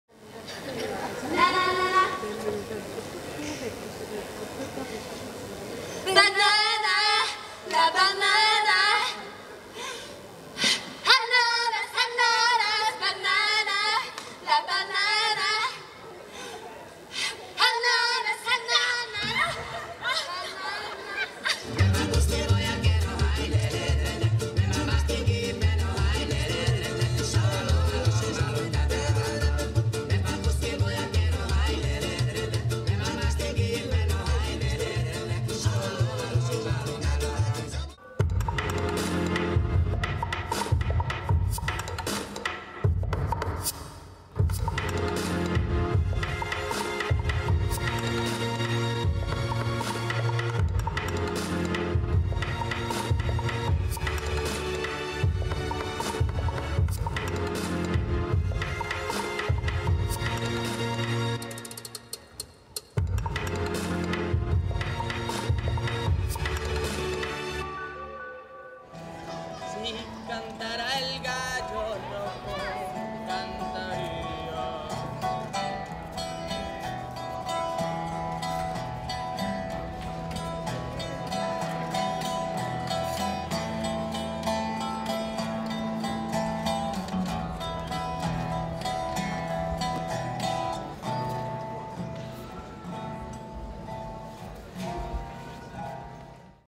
Ha començat el festival de dansa Agitart 2015 a Figueres
Tres dies dedicats a la dansa. Figueres es mou amb Agitart. Aquí teniu els fragments d'algunes actuacions que s'han vist el dijous 2 de juliol a la plaça de l'Ajuntament.